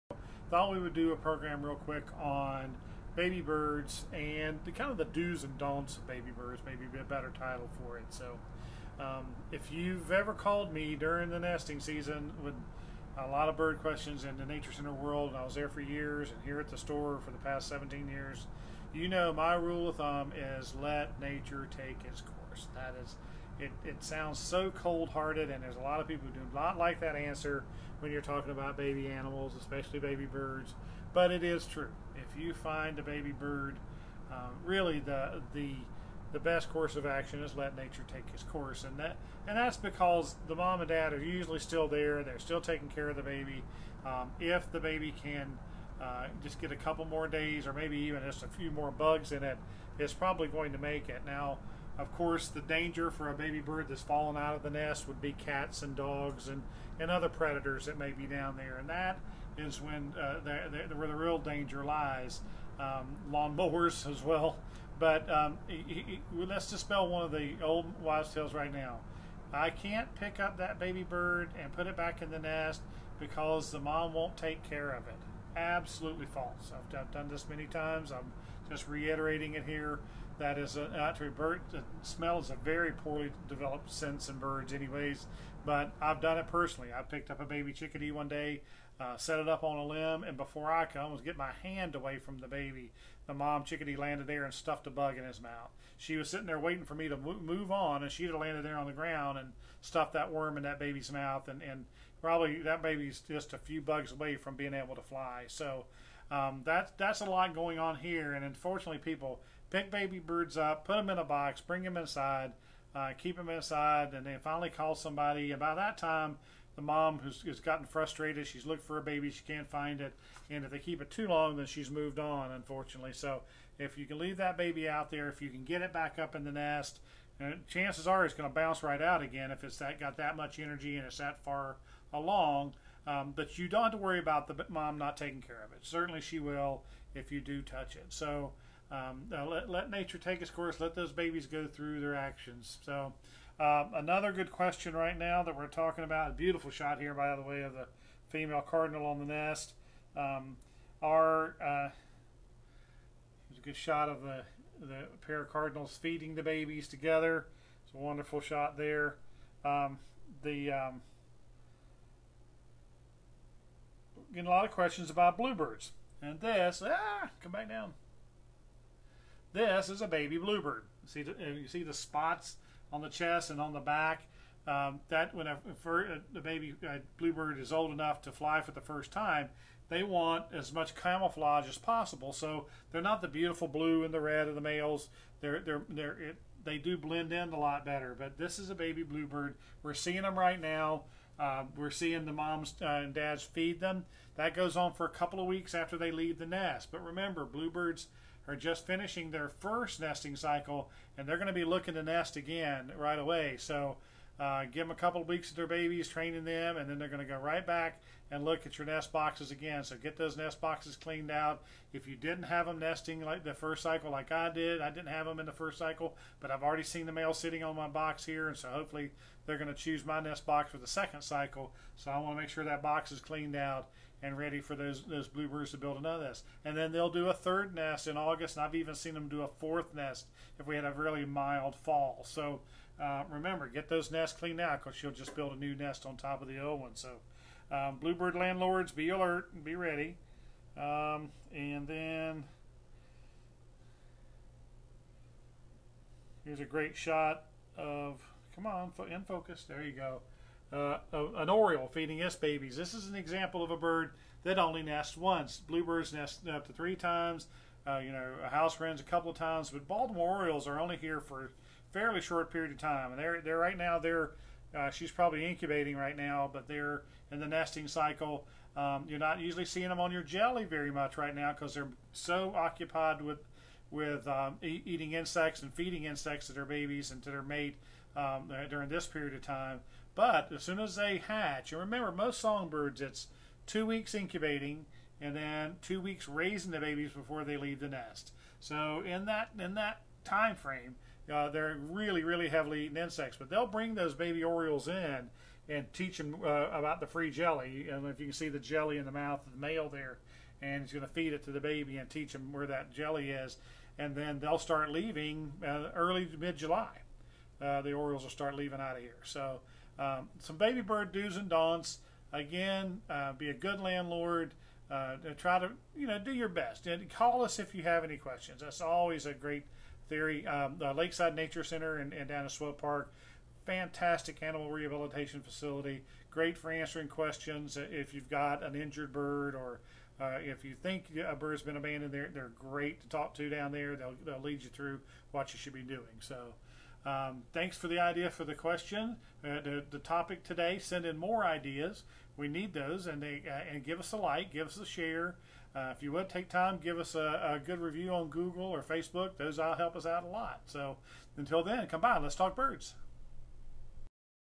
Baby Bird Season
Late Spring is the season for questions about baby birds.  Mark addresses some of the most common questions we get each year.

Baby Northern Cardinal picture by Marky Mutchler